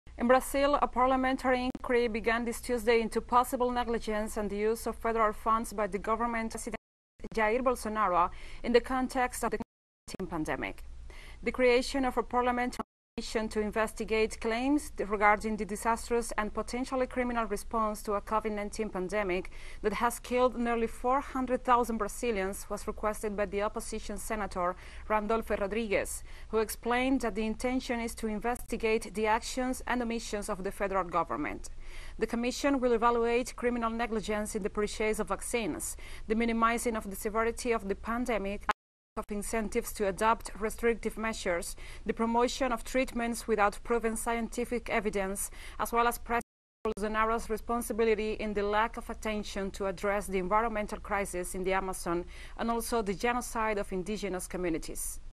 Brazil: Senate begins investigation on Govt handling of the pandemic
The Senate will scrutinize the government contracts and agreements that caused the transfer of resources to states and capitals and the crisis it unleashed such as requests for oxygen and other items in Amazonas and the summoning of the Minister of Health Marcelo Queiroga and the 3 ministers who left office. teleSUR